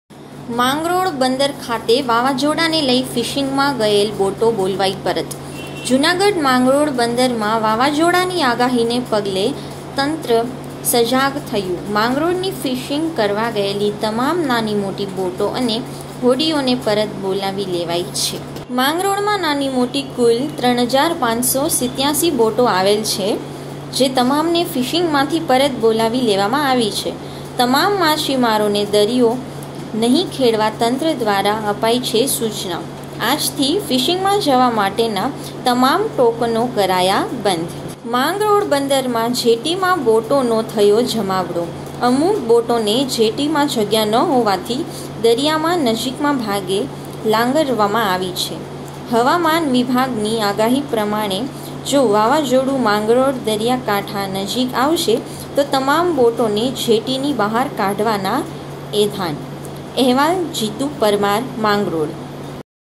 માંગરોળ બંદર ખાતે વાવાજોડાને લઈ ફિશિંગમાં ગયેલ બોટો બોલવાઈ પરત
જુનાગઢ માંગરોળ બંદરમાં વાવાજોડાની આગાહીને પગલે તંત્ર સજાગ થયું
માંગરોળની ફીસીંગ કરવા ગયેલી તમામ નાની મોટી બોટો અને હોડીઓને પરત બોલાવી લેવાઇ છે
માંગરોળમાં નાની મોટી કુલ 3587  બોટો આવેલ છે જે તમામ  ને ફીસીંગ માંથી પરત બોલાવી લેવામાં આવી છે

તમામ માછીમારોને દરીયો નહી ખેડવા તંત્ર દવારા અપાઇછે સુચના

આજથી ફીસીંગ માં જવા માટેના તમામ ટોકનો કરાયા બંધ

માંગરોળ બંદરમાં જેટીમાં બોટોનો થયો જમાવડો

અમુક બોટોને જેટીમાં જગ્યા ન હોવાથી દરીયામાં નજીકમાં ભાગે લાંગરવામાં આવી છે

હવામાન વિભાગની આગાહી પ્રમાણે જો વાવા જોડું માંગરોળ દરિયા કાંઠા નજીક આવશે તો તમામ બોટોને જેટીની બહાર કાઢવાના એંધાણ

*જીતુ પરમાર માંગરોળ*